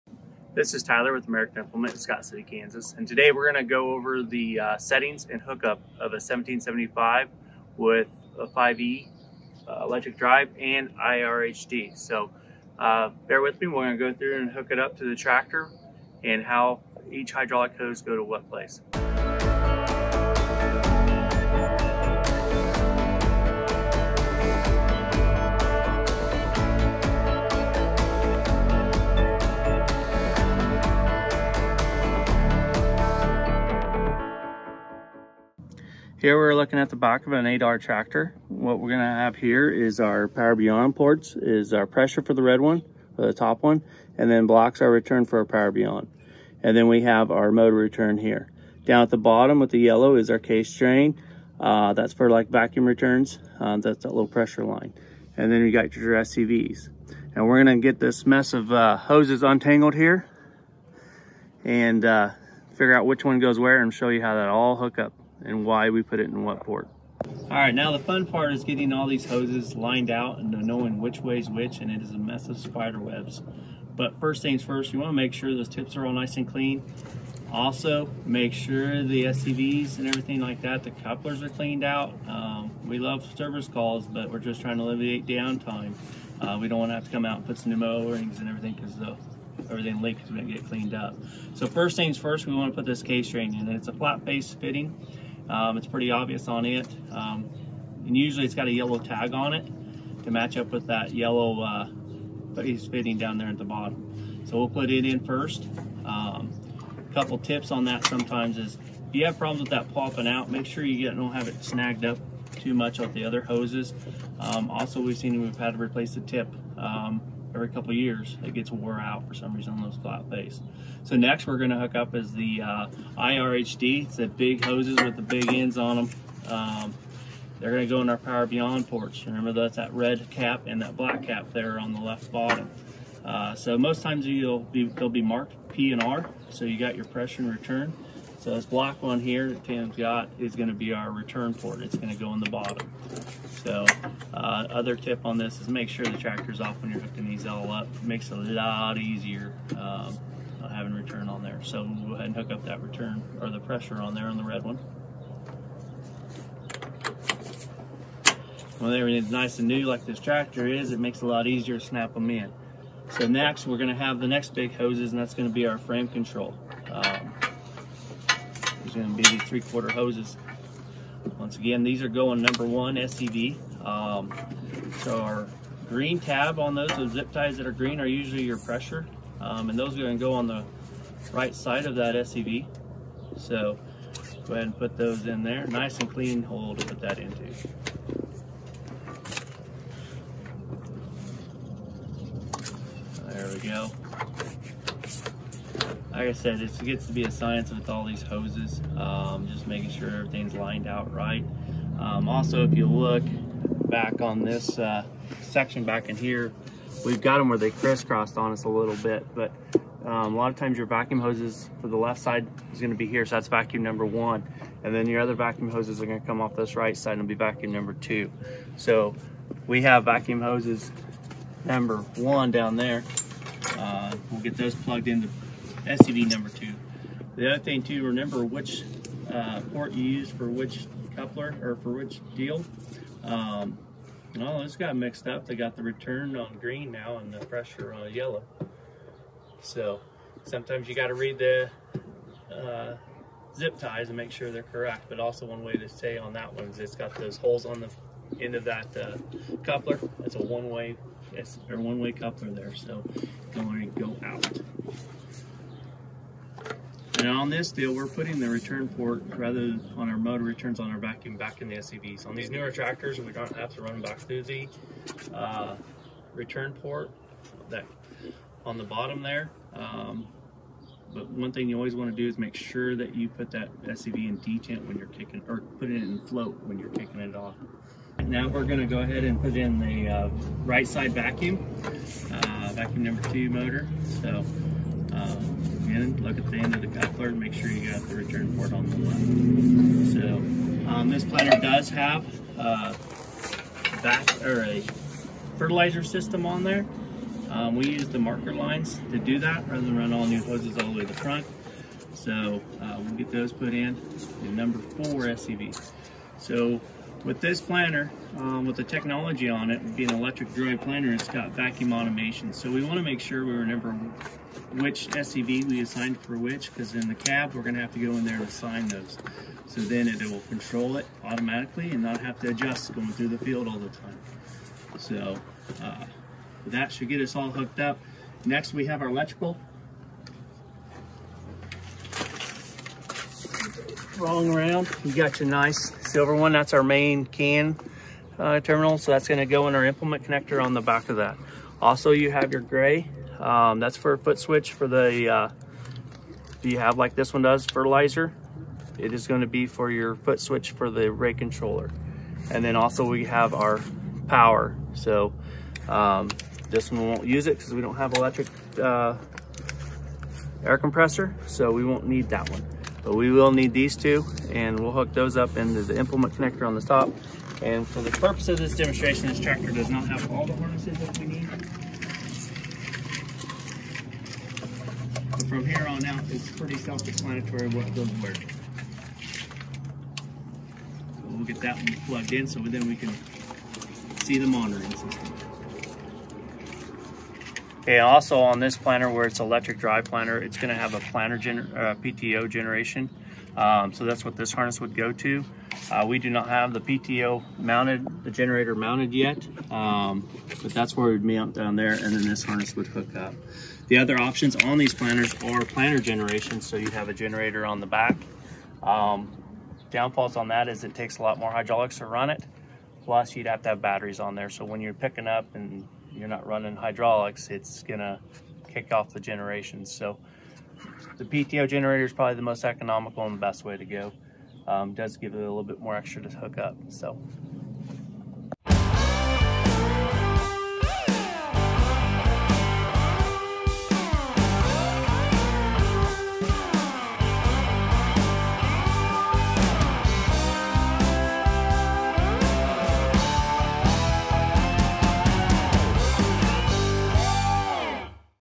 How To: A Complete Hookup for The John Deere 1775NT (5E) Planter. Part 1 - Power and Hydraulics
In this series Tyler Kough (Location Manager from Scott City, KS) demonstrates how to properly hookup a John Deere 1775NT planter. Part One deals with how to sort through and install the hydraulic hoses and power cables that will run your planters features.